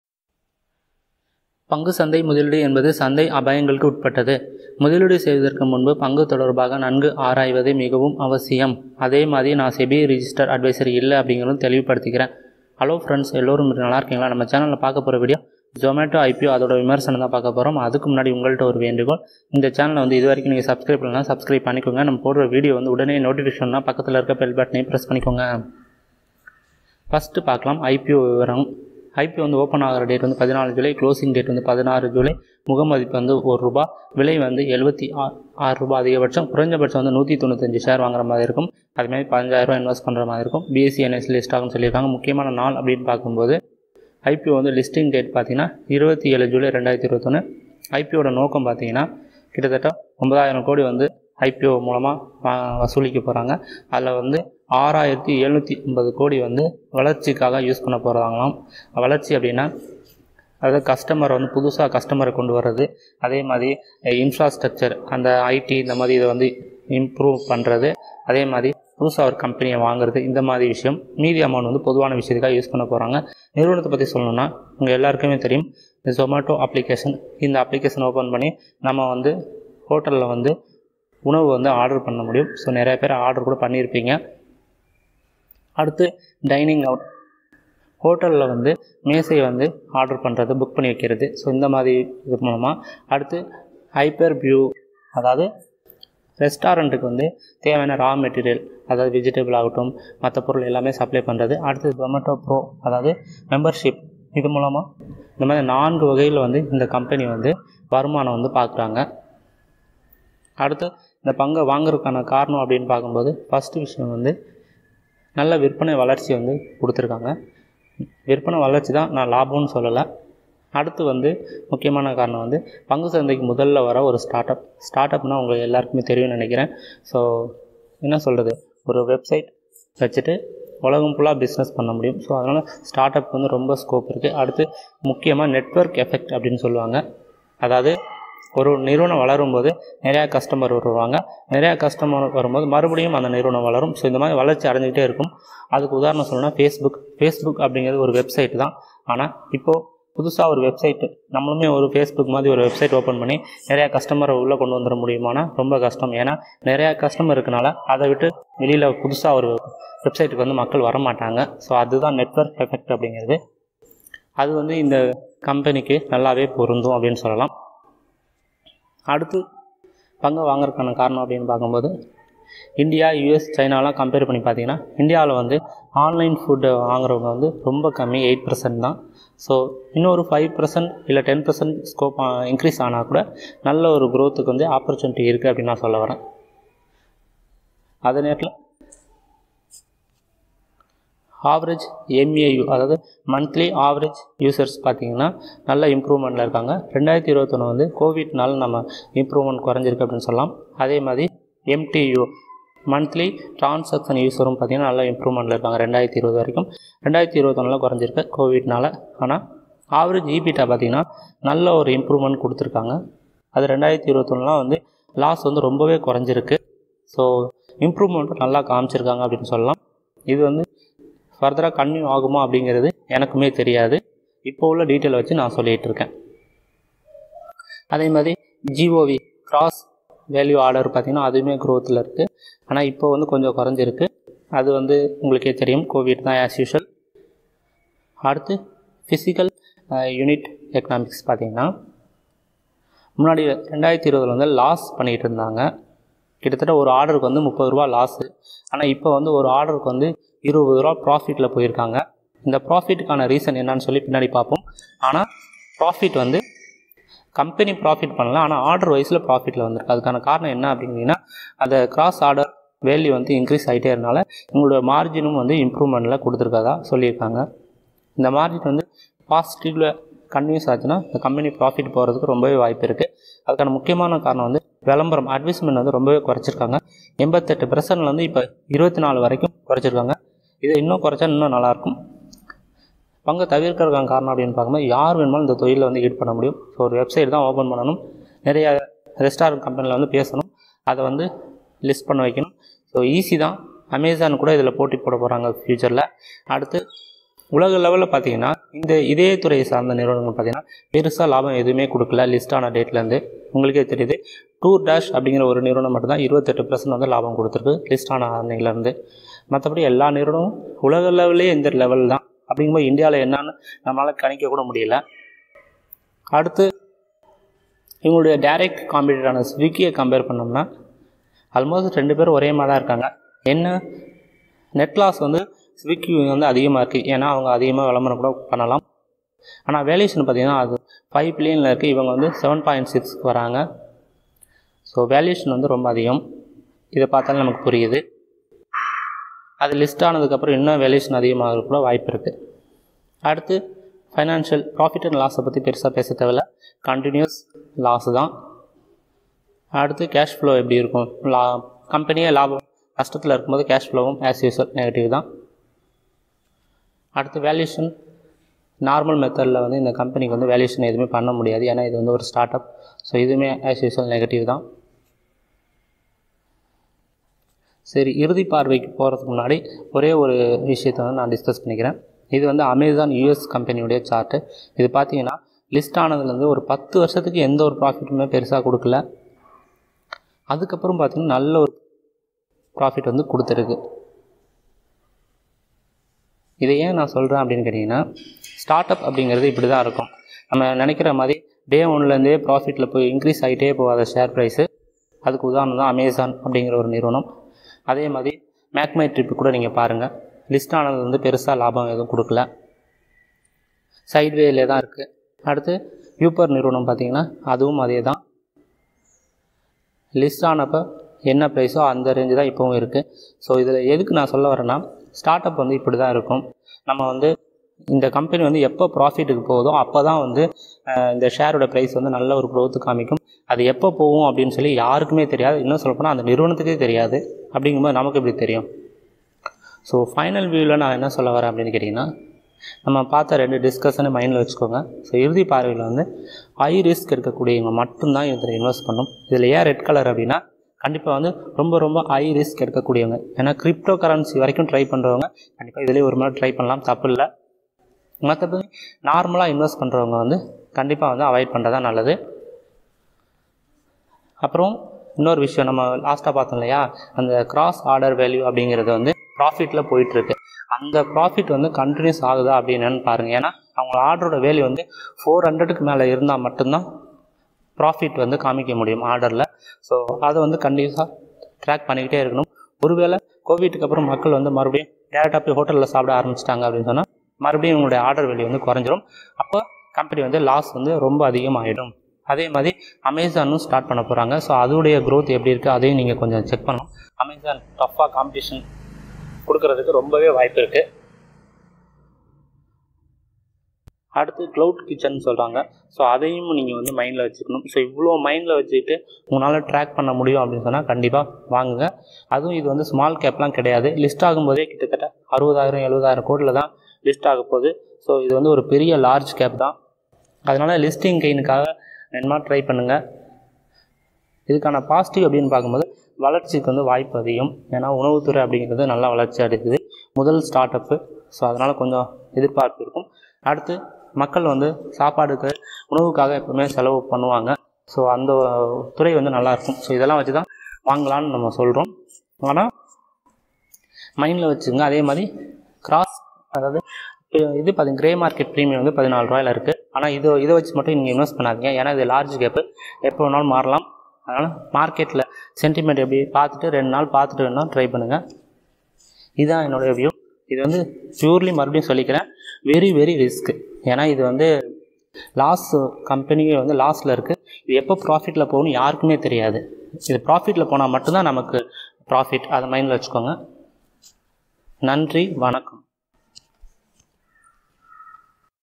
Zomato ipo review Tamil | Share market upcoming IPO
In this video gives information about upcoming ipo Zomato IPO details with financial information and Zomato  businiess overview.